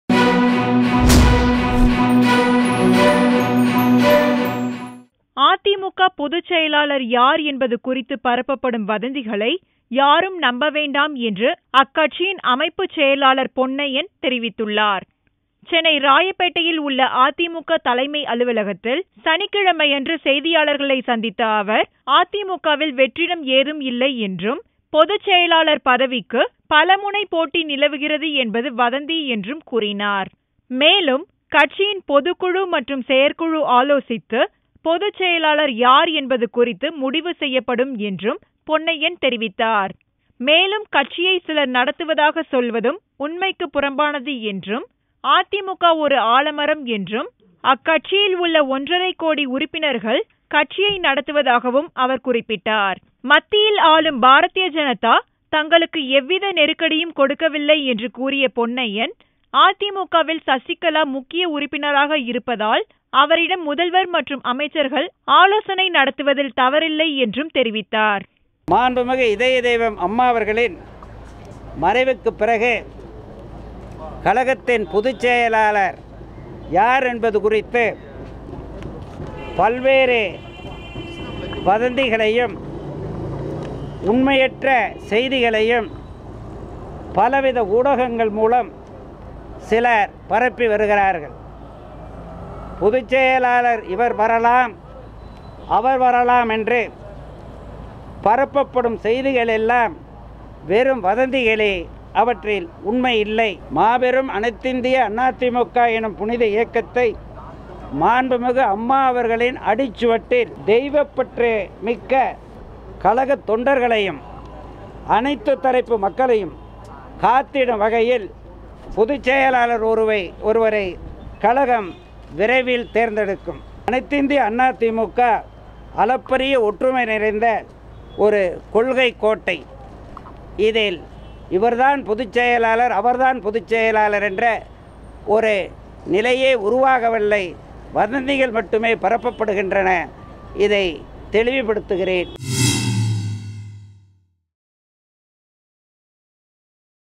Don't believe in rumors : C. Ponnaiyan
No infighting in AIADMK party ; don't believe in rumors : C. Ponnaiyan For more videos